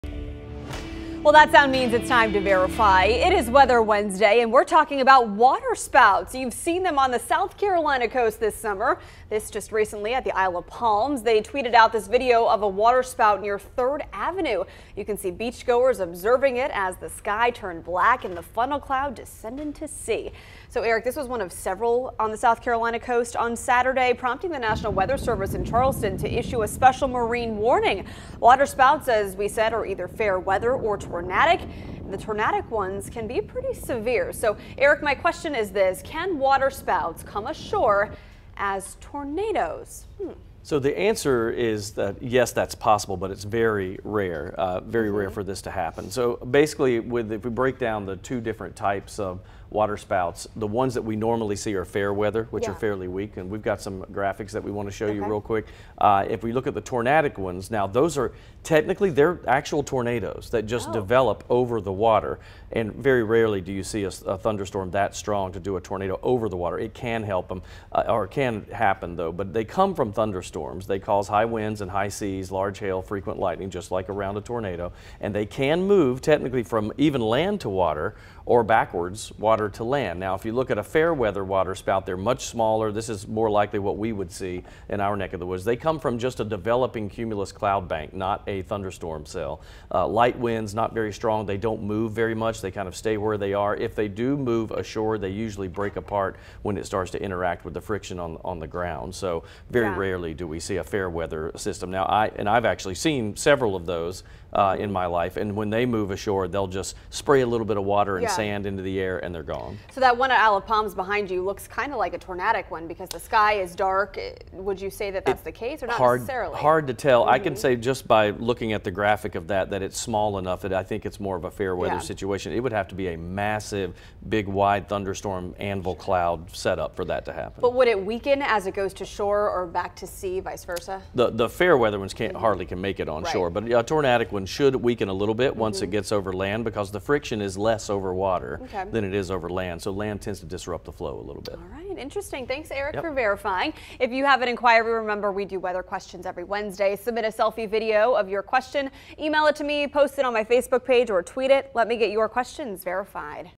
VERIFY: Yes, Waterspouts Can Come Ashore
You ask; we VERIFY. Waterspouts can move from sea to land, but they are typically weak, and most waterspouts carry no public danger.